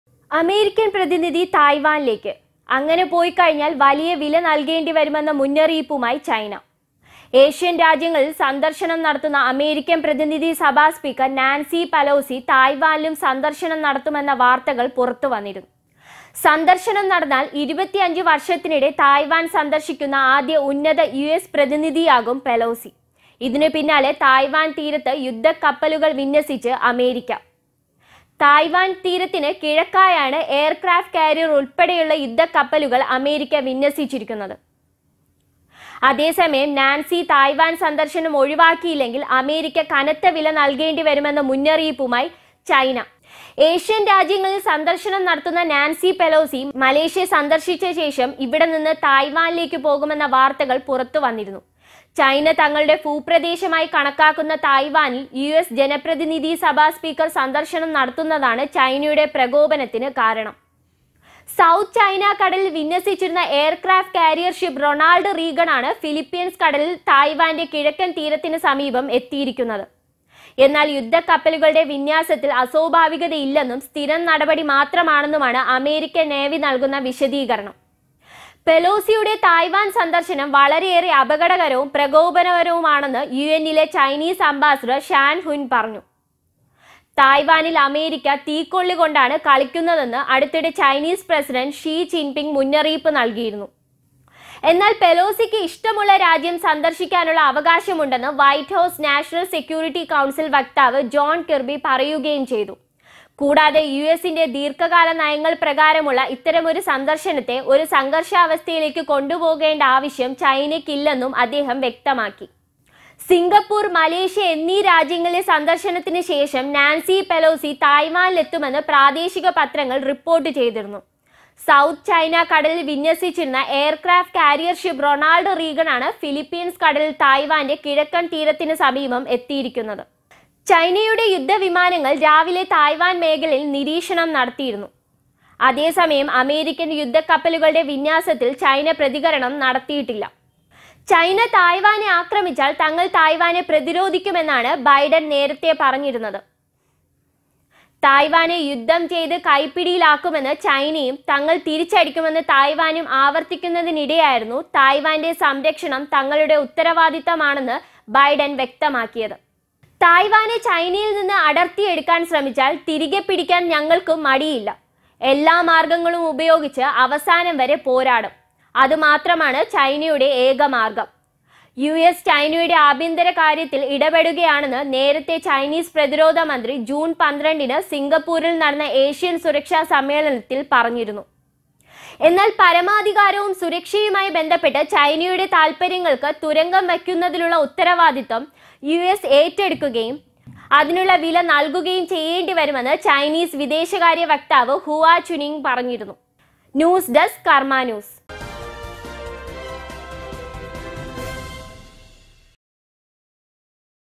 തായ്‌വാനിലെക്ക് അമേരിക്കൻ പ്രതിനിധി;വെല്ലുവിളിച്ച് ചൈന
അമേരിക്കൻ പ്രതിനിധി തായ്വാനിലെക്ക്; തായ്‌വാൻ തീരത്ത് പടക്കപ്പൽ വിന്യസിച്ച് അമേരിക്ക.